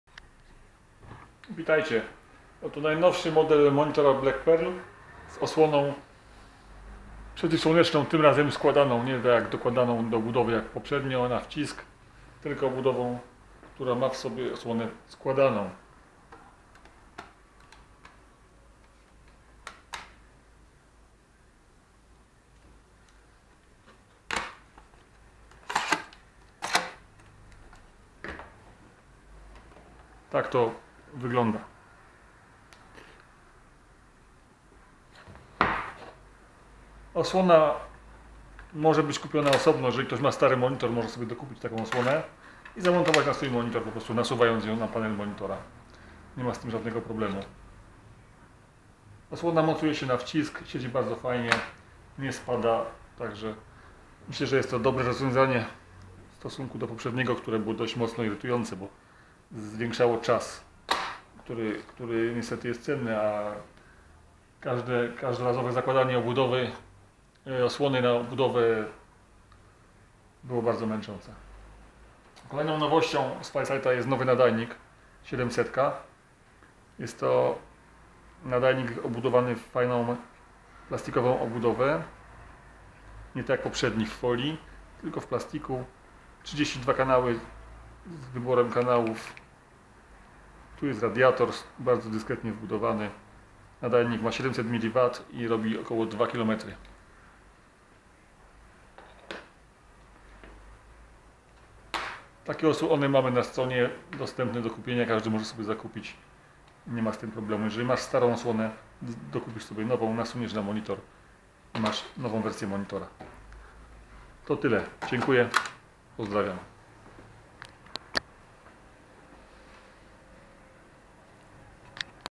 Newest version of Flysight Black Pearl RC801 monitor now with folded sun shade !
Hot new updated version of Black Pearl FPV monitor. Now available with folded sun shade ! New TX5807 700mW TX video transmitter .

New sunshields can be also bought seperately.